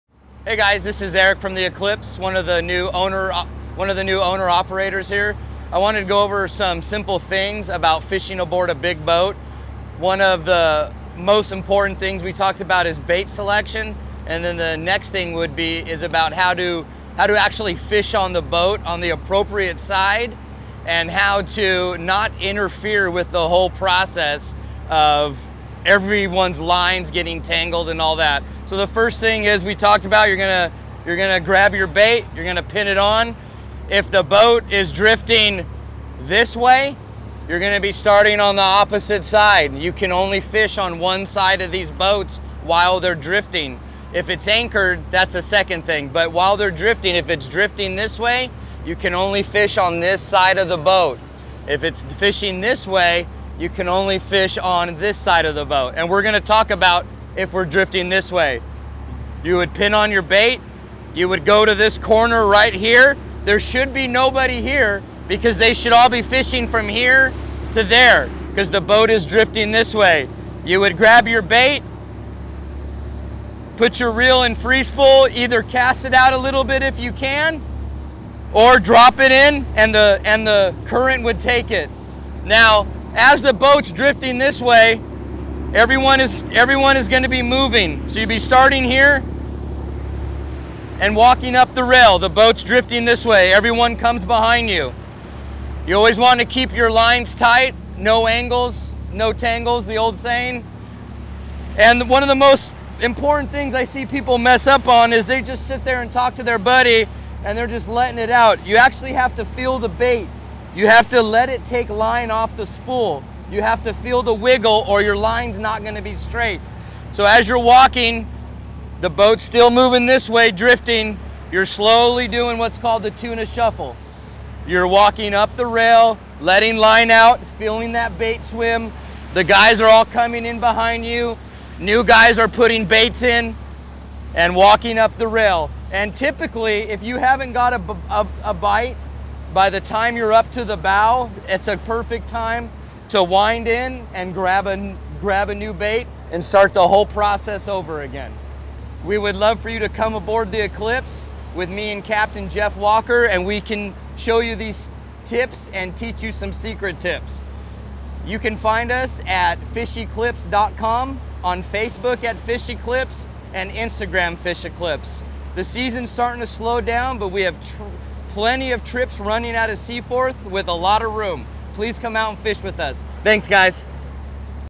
Live Bait Fishing Tips | SPORT FISHING
Capt. Eric from the Eclipse explains how to fish with live bait on a big sport fishing boat. Use this link to join Dan on a fishing charter,